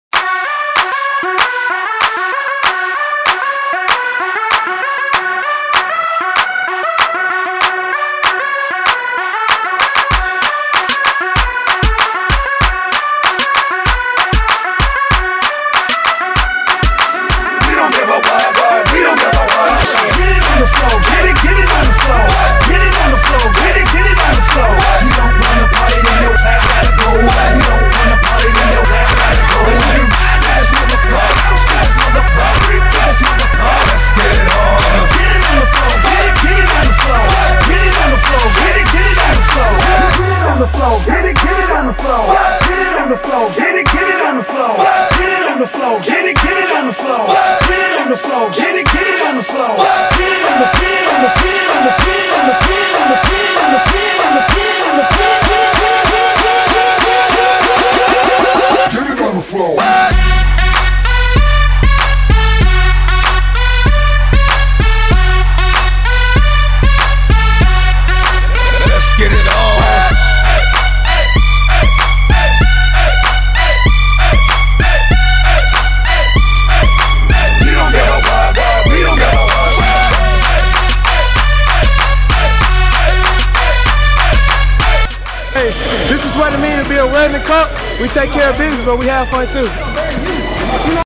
Colts Basket Ball Teachers Vs Student
Worthing Colts face off against the teachers in an epic clash of skill vs energy. Who will win?